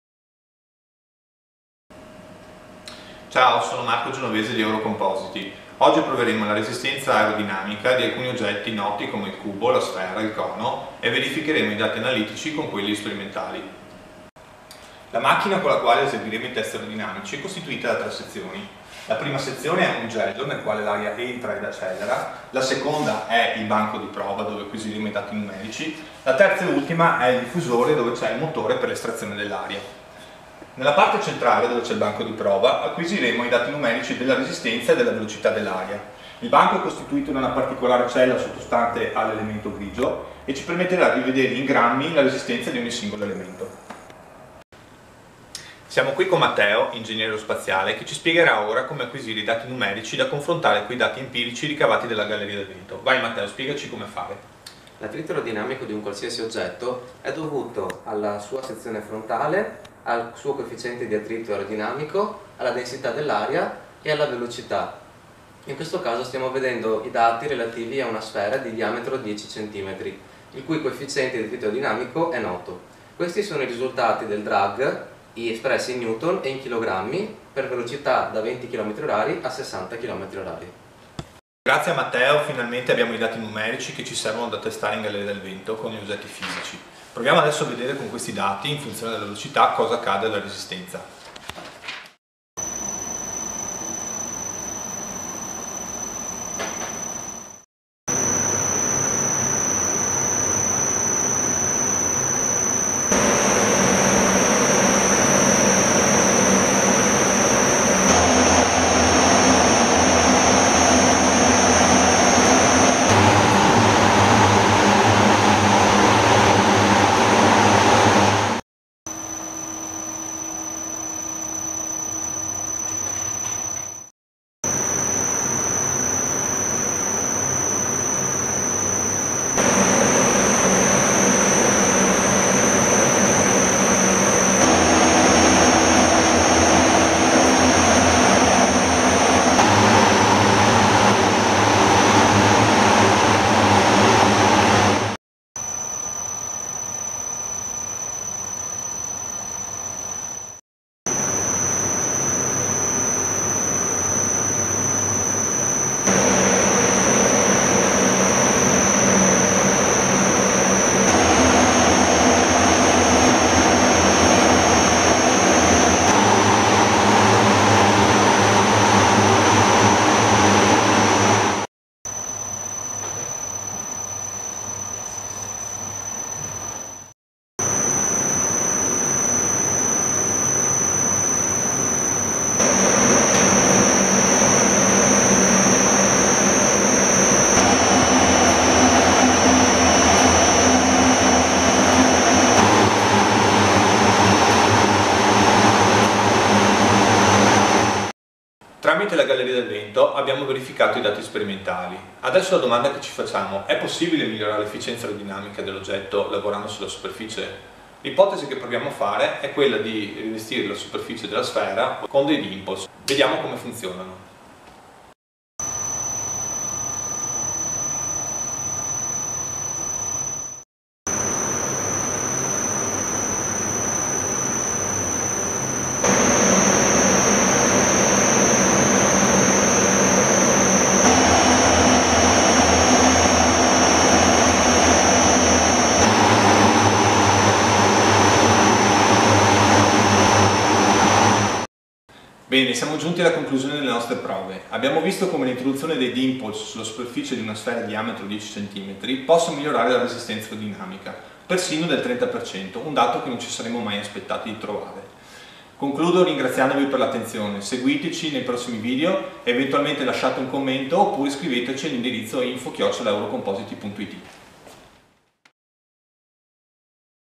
Wind Tunnel: verifica della resistenza aerodinamica di solidi noti
In questo video verifichiamo la resistenza aerodinamica di corpi solidi noti (sfera, cono, cubo, cubo inclinato) e mettiamo a confronto una sfera liscia con una coperta di dimples.